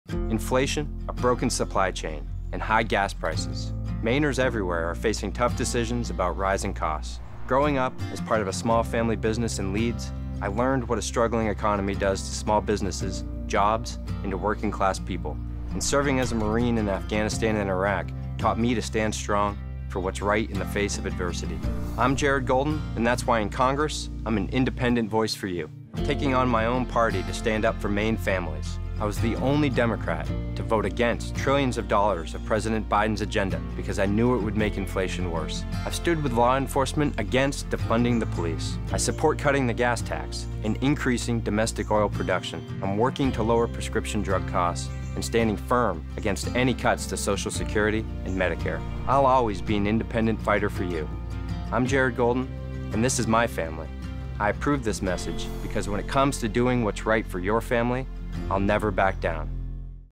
Independent | Jared Golden for Congress (ME-02)
Growing up in a small family business, Jared saw what a struggling economy does to small businesses, jobs, and to working-class people. In the Marines, he learned to stand strong for what's right. That's why in Congress, Jared is an independent voice for you, taking on his own party to stand up for Maine families.